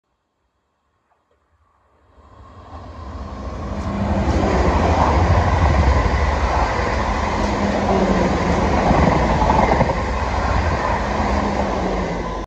Class 444 Desiro | 444020 + 444008 | Sparking | South Western Railway
A pair of 444’s speed through with lots of sparks.